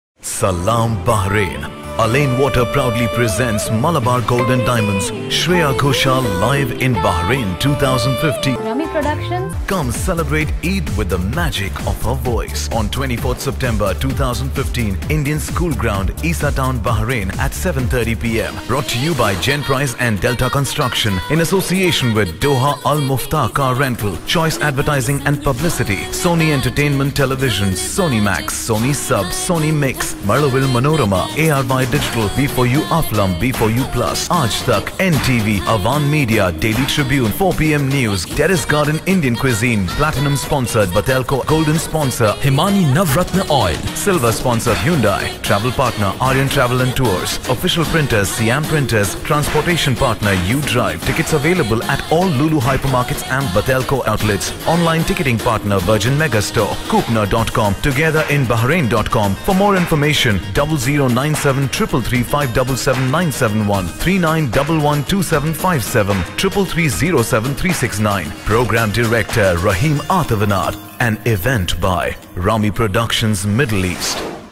Salam Bahrain Shreya Ghoshal Live in Concert 2015
The Indian School, Bahrain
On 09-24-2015
At 7:30 PM
An Event By: Rami Productions Middle East
Program Director: Raheem  Athavanad